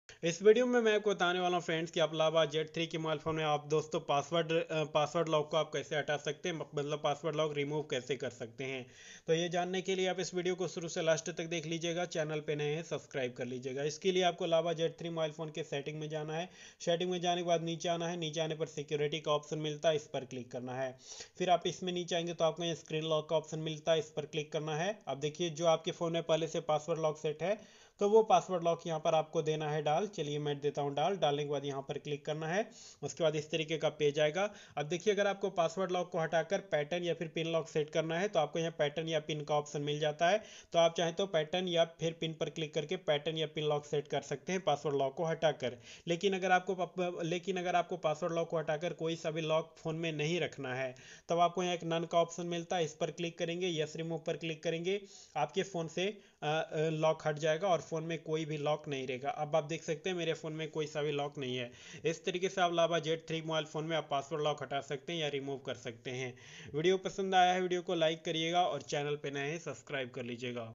lava z3 me password lock kaise hataye।how to remove password lock in lava z3।lava z3 password lock
is video me bataya gaya hai friends ki lava z3 phone me password lock kaise hataya jata hai ya remove kaise kiya jata hai